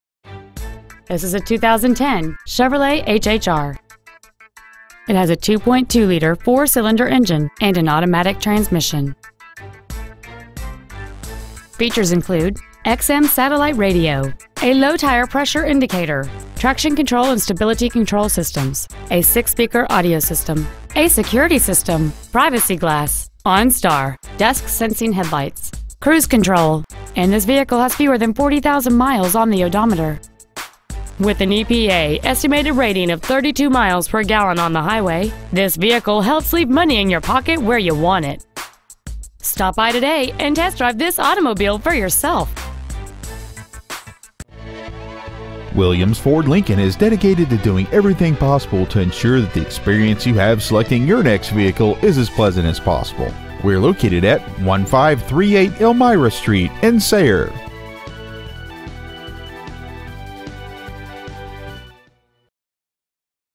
2010 Chevrolet HHR Sayre PA
We have been honored to serve the Sayre PA area , we promise that your experience at our dealership will exceed your expectations !

 Year : 2010
 Make : Chevrolet
 Model : HHR
 Engine : 2.2L I4 16V MPFI DOHC FLEXIBLE FUEL
 Trans . : AUTOMATIC
 Exterior : BLUE
 Miles : 39,040

 Stock : FTP152